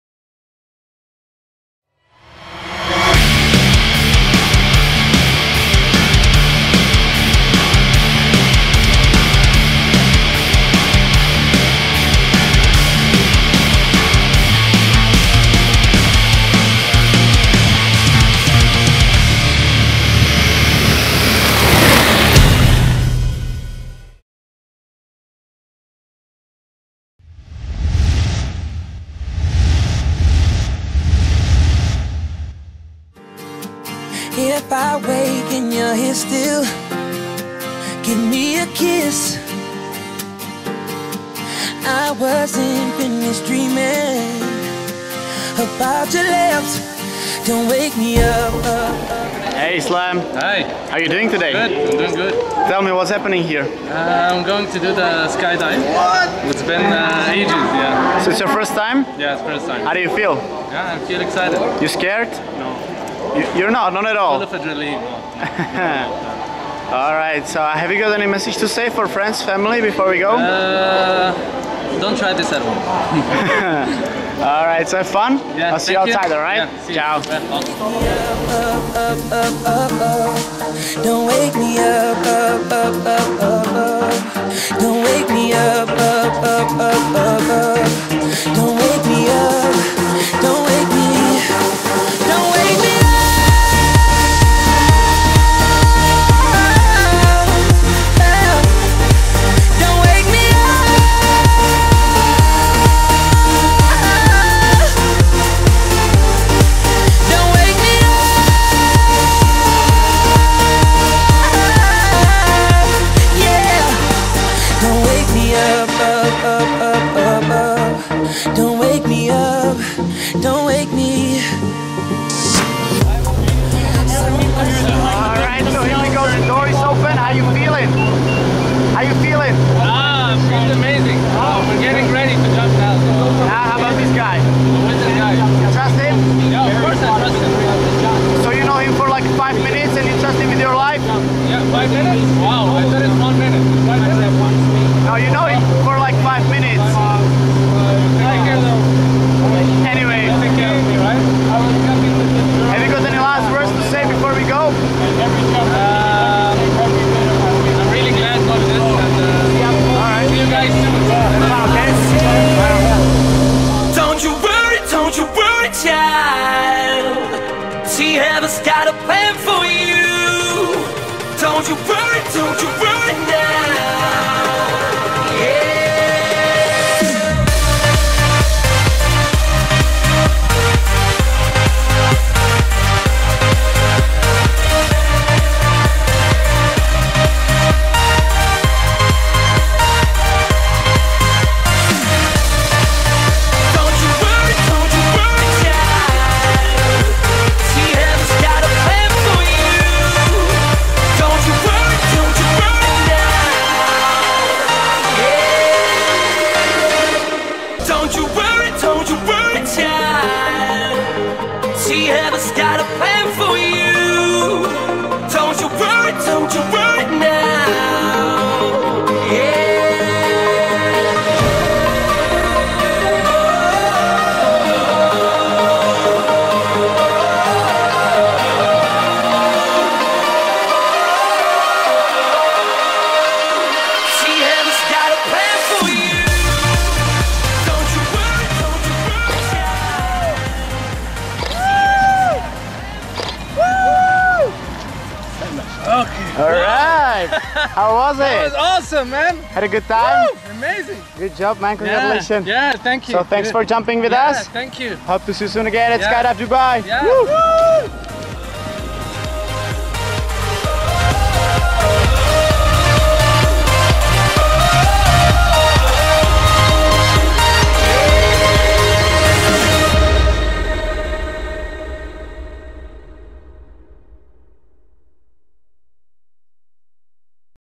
A life time Experience ...Sky Diving Over the Palm. Dubai 2013
Me Sky diving in dubai over the Palm  - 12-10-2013
Islam Fayez , Dubai